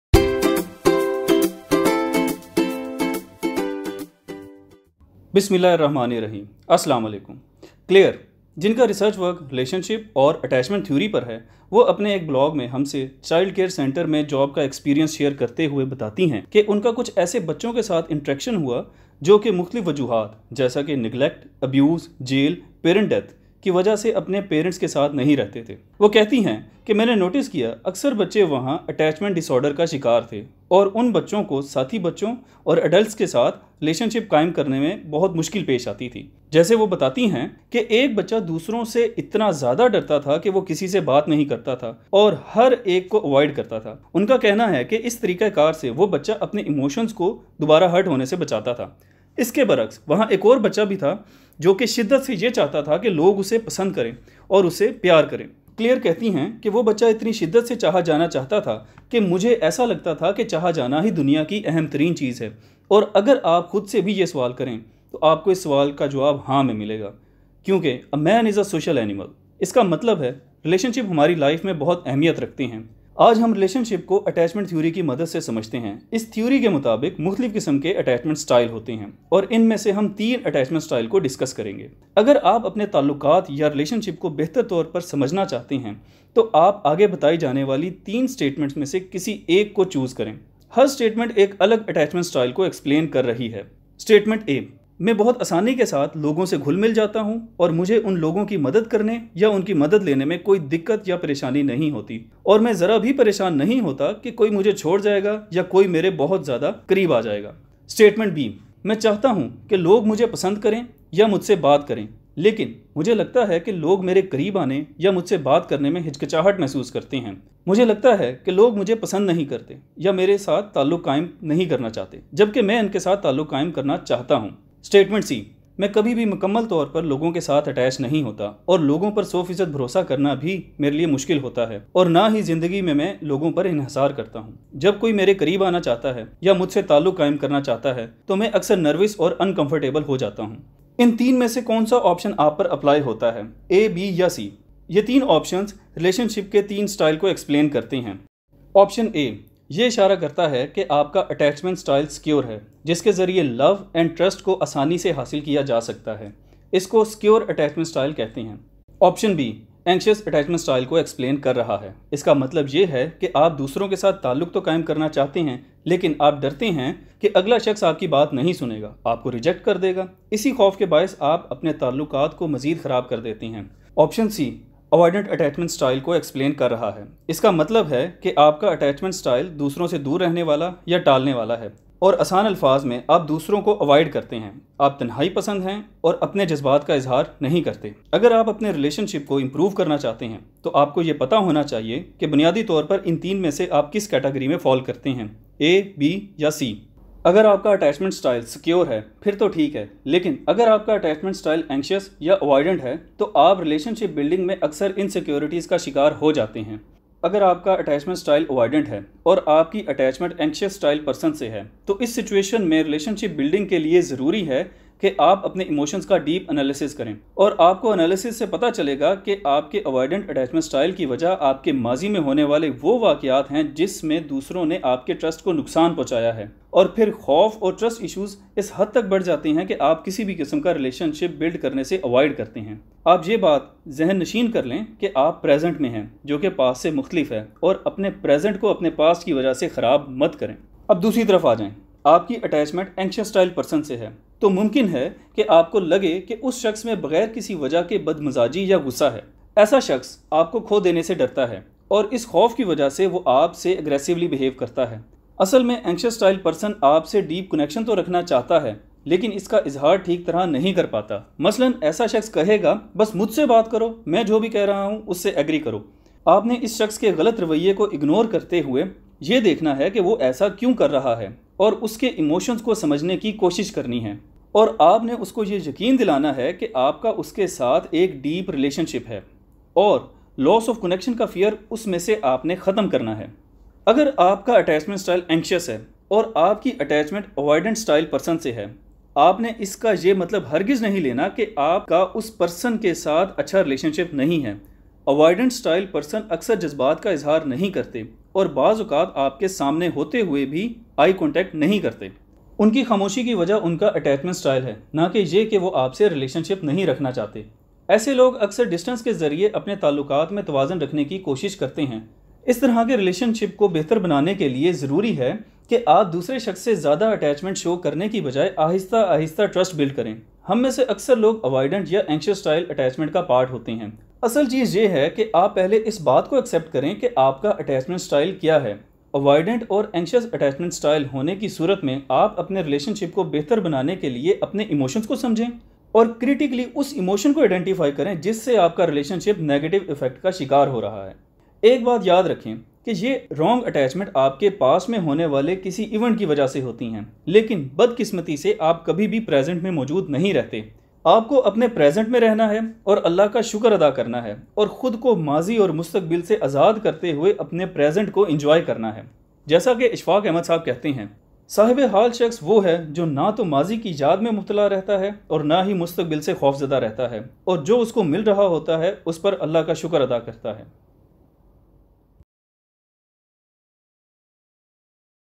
Relationships (Attachment Theory and living in Present) Urdu
A very Simple video to understand about the Relationships and Attachment styles.

Urdu: Is video main hum discuss krain gay k app kaisay apnay taaluqaat ko bhter bnasktay hain. Maazi aur mustaqbil mian rhany ki bajaey app haal mian rhtay howay apnay taaluqaat ko mzeed ghra bnayain.

1. Secure Attachment Style
2. Avoidant Attachment Style
3. Anxious Attachment Style
4. Past Present and Future